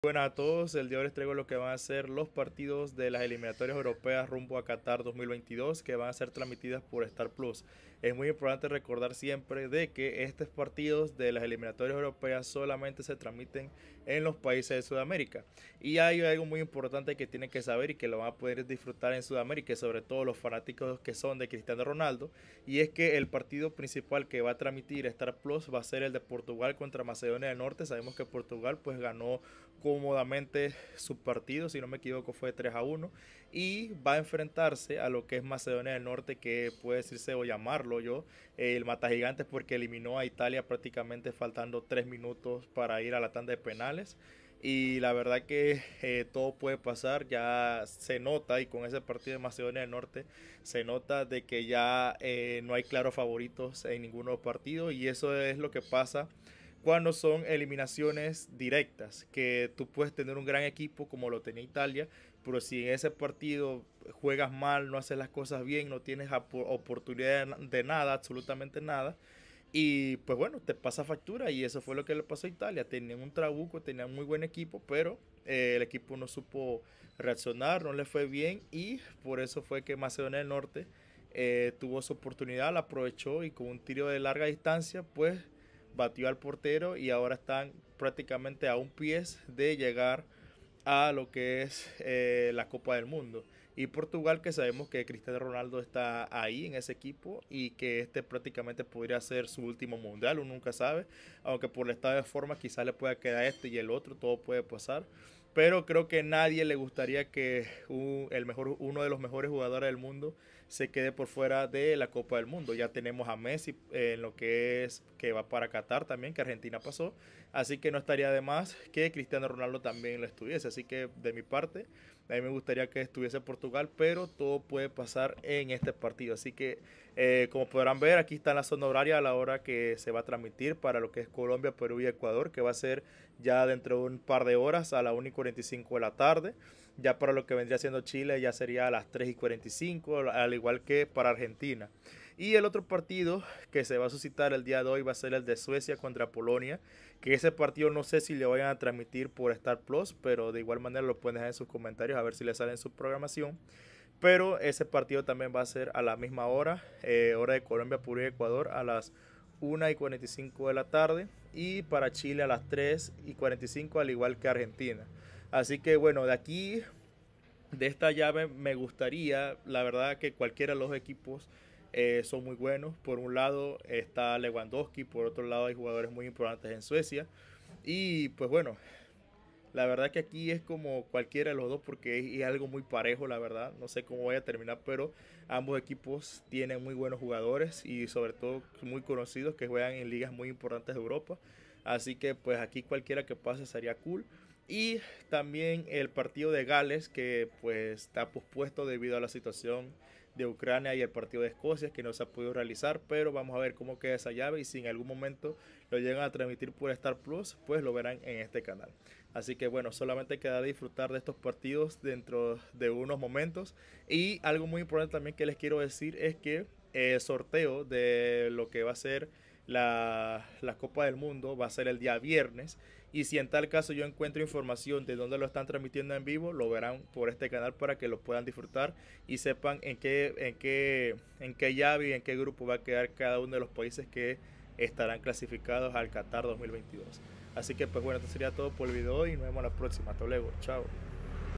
STAR+| Partidos EN VIVO de las Eliminatorias Europeas Qatar 2022 del 29 de marzo del 2022
Estos serán los partidos de las eliminatorias europeas que serán transmitidos por STAR+. Es importante recordarles que estos partidos solo se podrán ver en STARPLUS de Sudamérica.